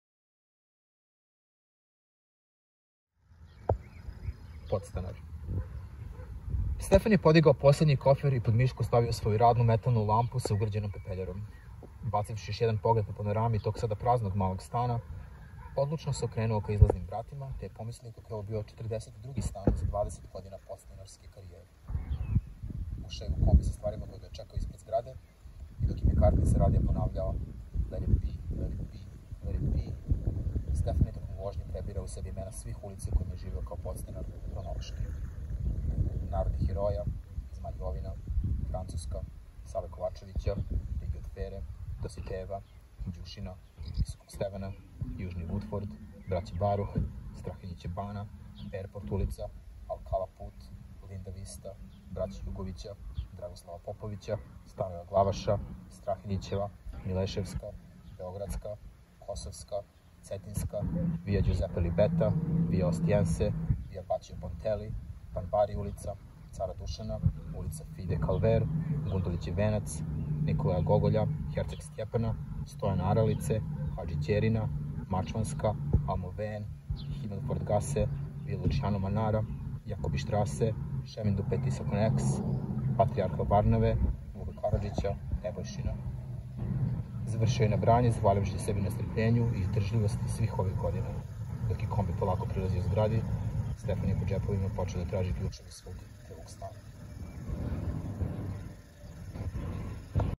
Kratka priča Podstanar iz knjige "Divertimento za četiri godišnja doba"
Kratku priču Podstanar čita Luka Ivanović iz knjige priča "Divertimento za četiri godišnja doba" , Danice Radovanović. Knjiga sadrži 100 kratkih priča, isto toliko, i više, paletu zanimljivih junakinja i junaka. Može se čitati nelinearno.

i ne zaboravite: čitanje spasava i oplemenjuje!